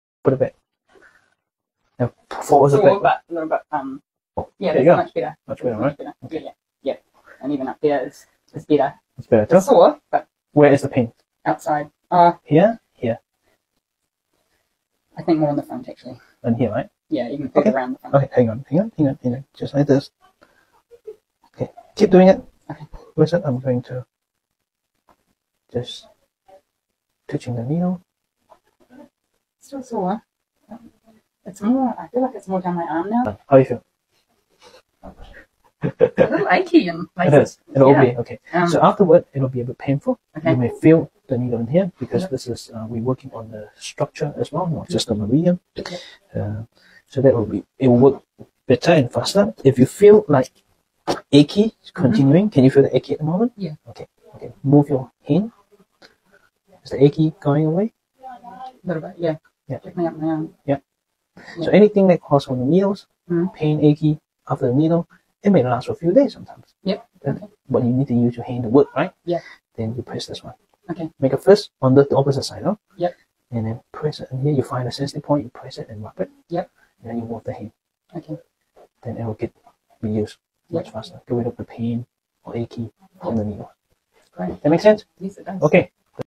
Effective Post-Acupuncture Care: Managing Soreness and Residual Sensations! Embark on your healing j
Effective Post-Acupuncture Care: Managing Soreness and Residual Sensations! Embark on your healing journey with confidence as our expert acupuncturists provide you with comprehensive aftercare instructions. Discover the subtle yet profound post-treatment sensations and learn how to alleviate them with ease. Following your acupuncture session, a mild soreness or lingering needle sensation is natural. Here are our specialists’ advice for a comfortable recovery: 1. Gentle Massage: Apply a soft, soothing pressure around the affected area to enhance circulation and help disperse any built-up tension. 2. Shake It Off: Lightly shake the limb or area where you feel the residual needle sensation. This can stimulate blood flow and help reduce any discomfort. Remember, these sensations are typically mild and fade away shortly. However, should you experience any significant discomfort, don‘t hesitate to contact your acupuncturist for further guidance. Your well-being is our top priority, and we are committed to ensuring a tranquil and healing experience for every patient.